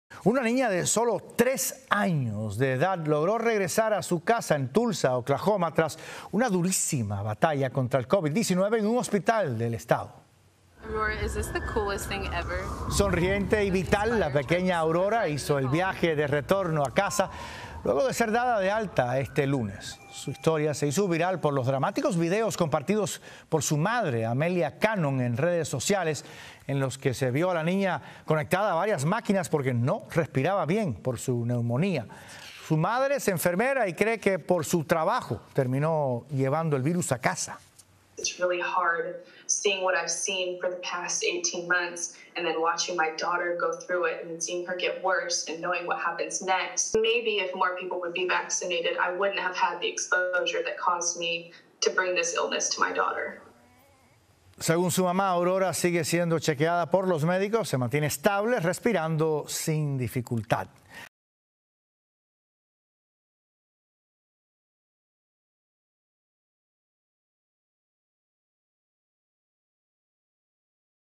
Niña vuelve a casa tras ganarle la batalla al COVID-19 | Noticias Telemundo
Video oficial de Noticias Telemundo. La pequeña Aurora logró volver a su casa en Tulsa, Oklahoma, luego de casi perder la vida por el COVID-19. Su historia se hizo viral por los dramáticos videos desde el hospital compartidos por su madre, Amelia Cannon, quien además es enfermera.